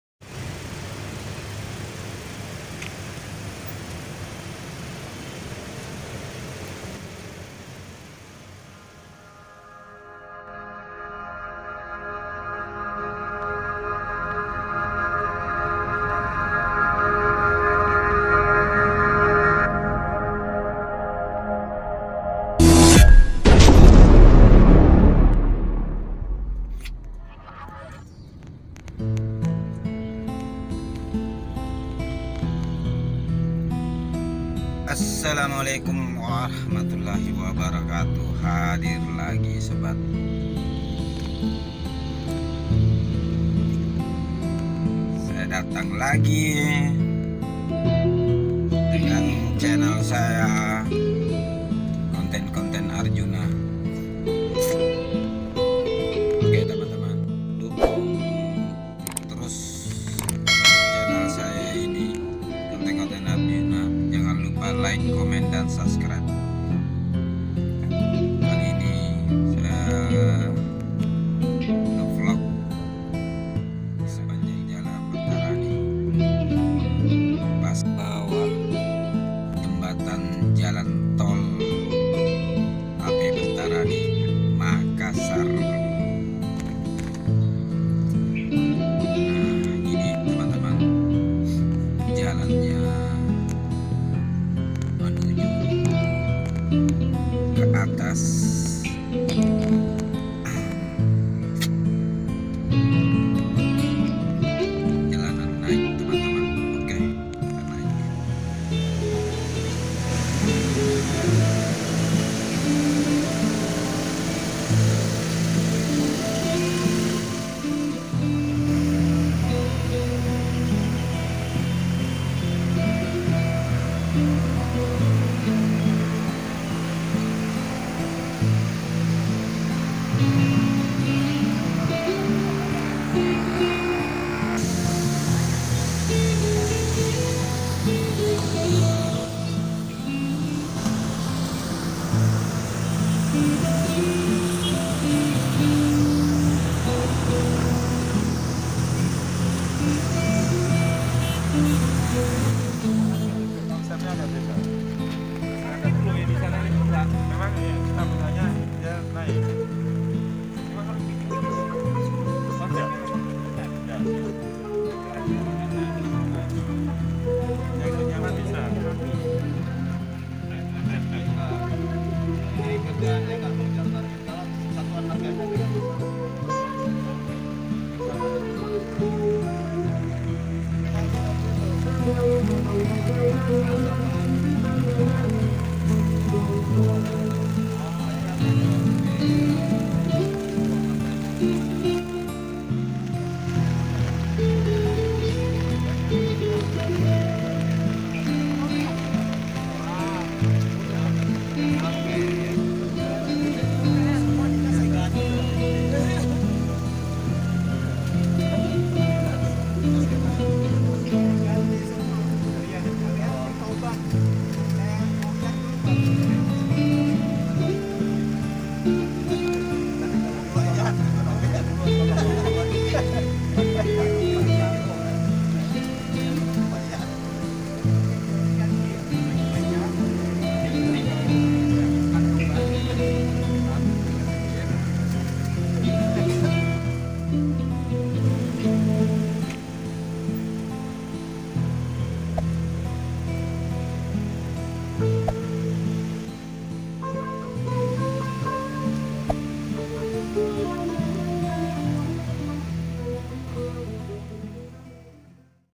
HEBOH!!! Anggota Komisi VI DPR RI Buka jalan,Jembatan Tol Layang Mks sdh bisa dilewati kendaraan
Kok bisa ya?
Jembatan tol layang Makassar sdh bisa kendaraan lewat pada hal belum diresmikan.
Penasaran ya???
Tonton skr videonya teman2 apa yg terjadi...
biar teman anda ikut penasaran..
Ok semoga terhibur
Terima kasih

Mariki' dii ...